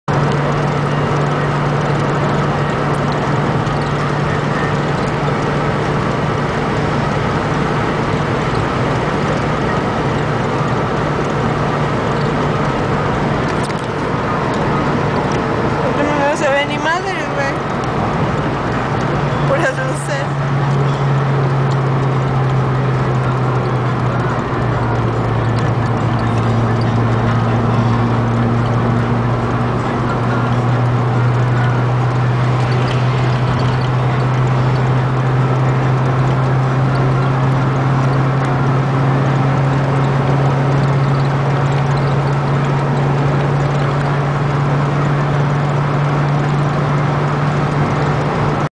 Carretera Salamanca - ceLaya
No se ve Ni MaDrEs Weeey! Grabado a las 3:30 am del domingo 18 de mayo 2008